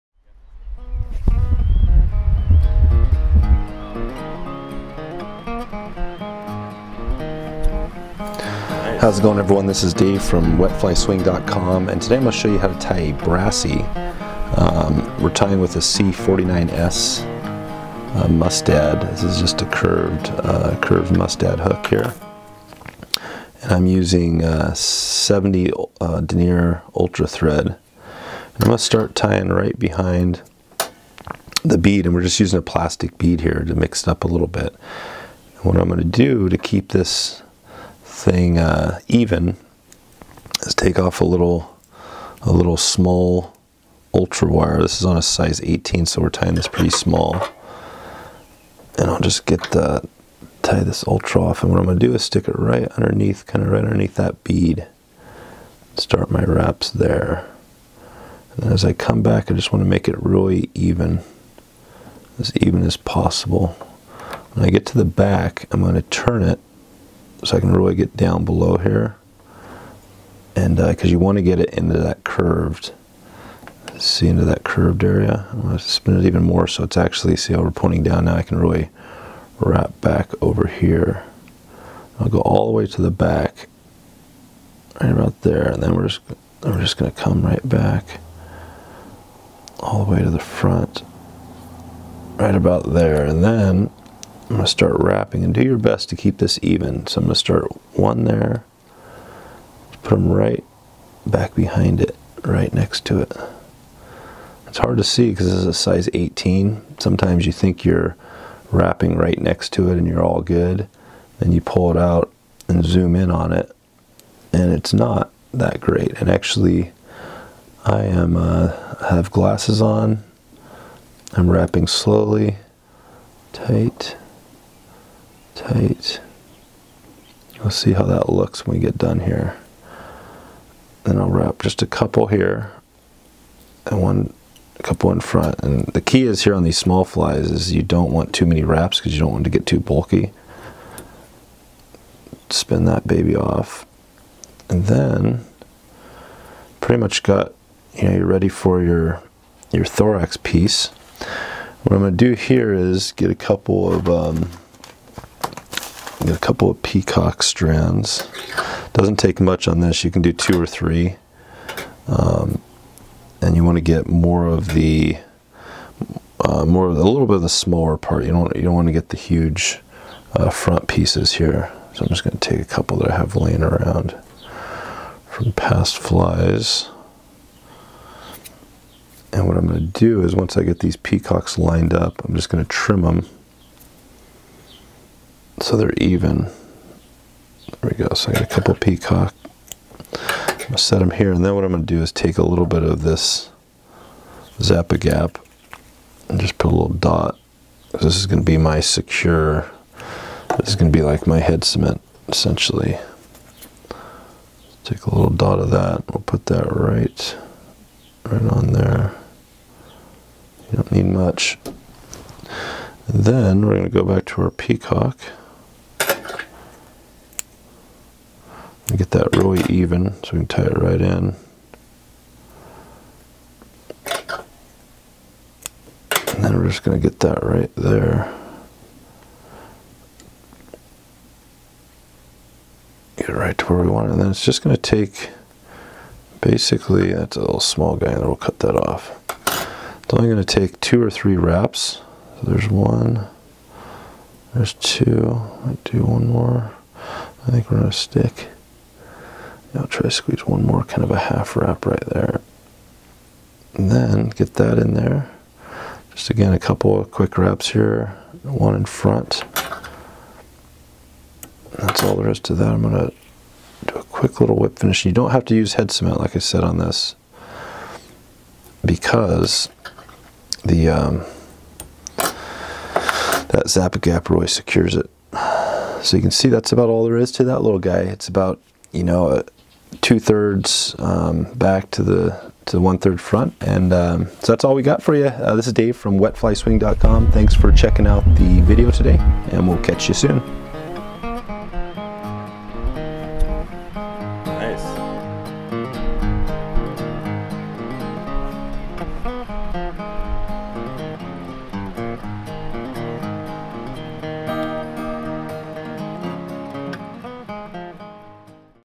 How to Tie a Brassie - Fly Tying Tutorial
In todays video I show you how to tie the Brassie which is a classic nymph that everyone should have in their box in various sizes and colors.

This specific pattern will have a pink bead and is in a size #18.

We are moving into season 2 of the podcast with a trout focus.  Click on the links below to listen to the show.